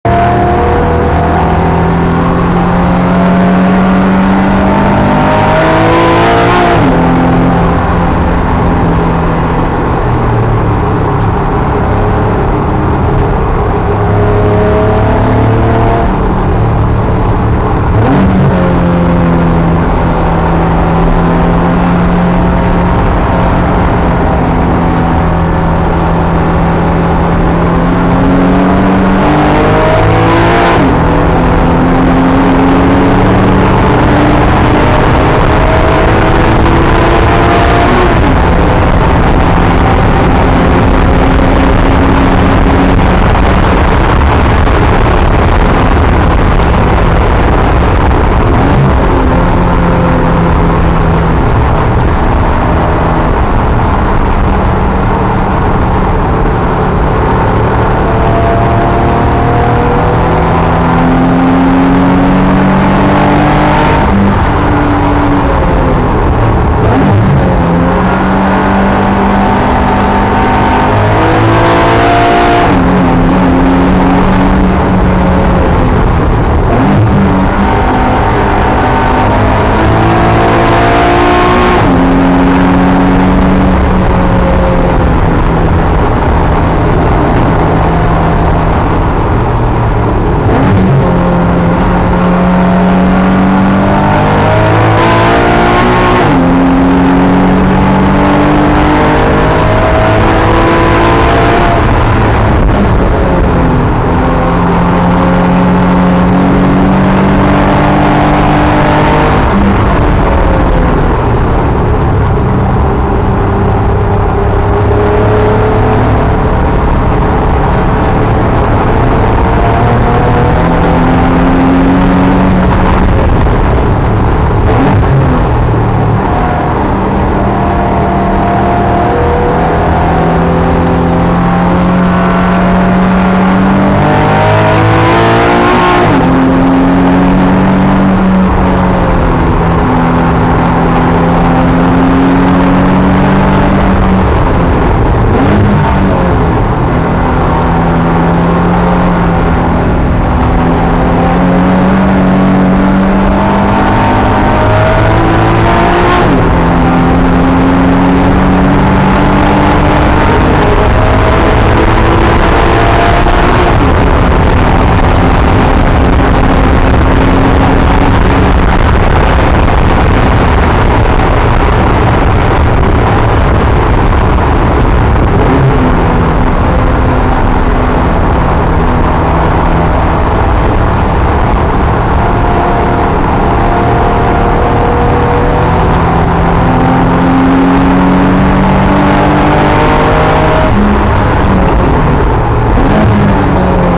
TWS CCW Jan26
Driver's Edge, Yellow Group, 3rd Session Sunday - Car #307 - Grey 370Z.  Best time I made this weekend at 2:06.  Just switched to a new wheel setup (18x10 square) and new tires (Hankook RS3).  I was very slow Saturday while learning to feel/trust the new setup and rubber (and breaking them in from brand-new).  Sunday finally things were clicking enough to push it a bit in the mid-corner and exits, but I was still being shy about braking late/hard enough.  The RS3's feel much better than my old Conti DW's.  I thought the traction would be trickier to manage on them, but it's actually much easier once I got a feel for it.  They give way in a much more predictable and linear fashion, and they don't get greasy at the end of a hard session like the old tires did.  Still, it will probably take another full weekend or so to finish adapting my brain to how it feels.